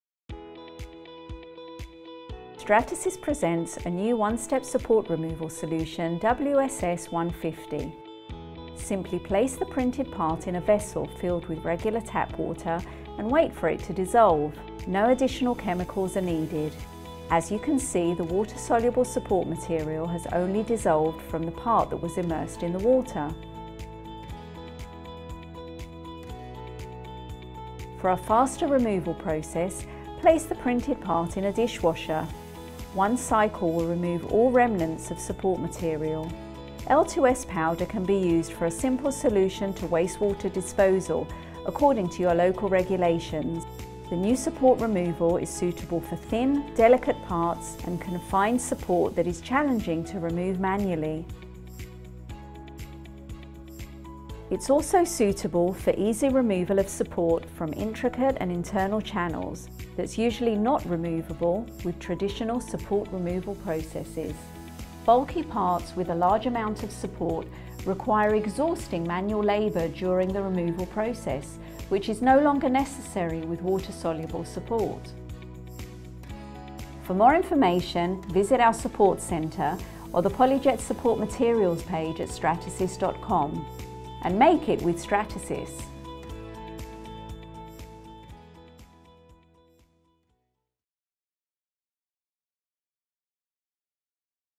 WSS™150 Water Soluble Support Material
The new WSS150 Water soluble support is for fast, hands-free removal with a standard or countertop dishwasher, or by submerging in a vessel filled with regular tap water (preferred solution for delicate and intricate parts with internal cavities). You can also use L2S™ powder for a simple solution to wastewater disposal.

Looking for more information?